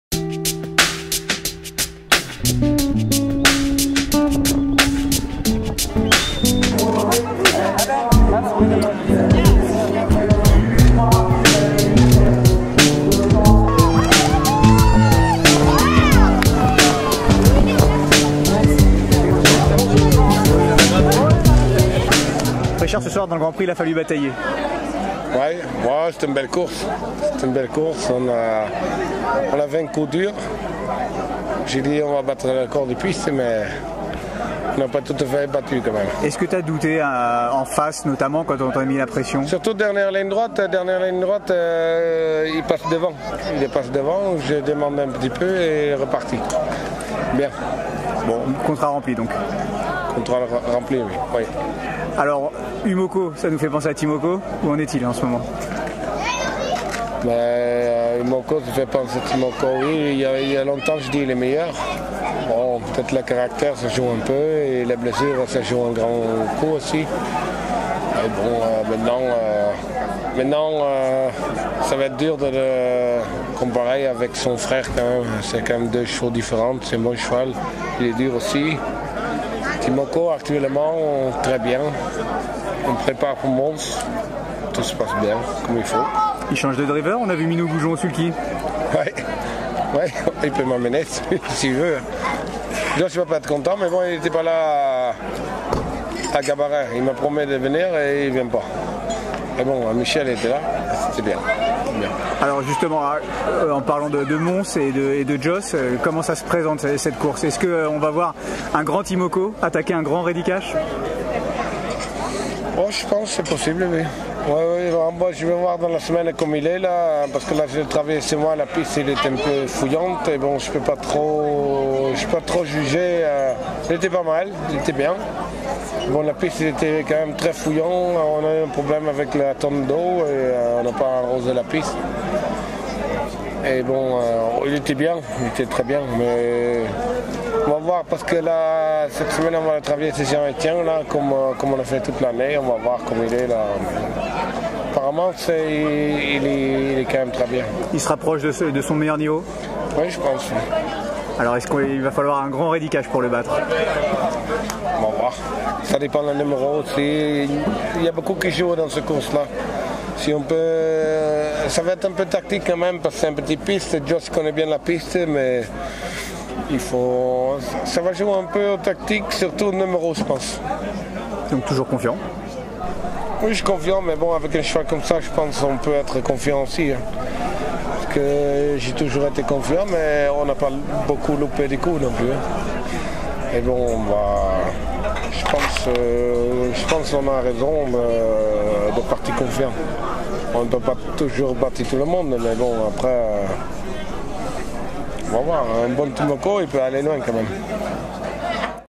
TROT - Des nouvelles de TIMOKO - UMOKO, lauréat du Px Président de Folleville à Biarritz
Après son succès dans le Prix du Président de Folleville avec UMOKO, Richard WESTERINK nous a donné des nouvelles de son champion TIMOKO qui se prépare pour le Grand Prix de Wallonie le 11 août prochain à Mons.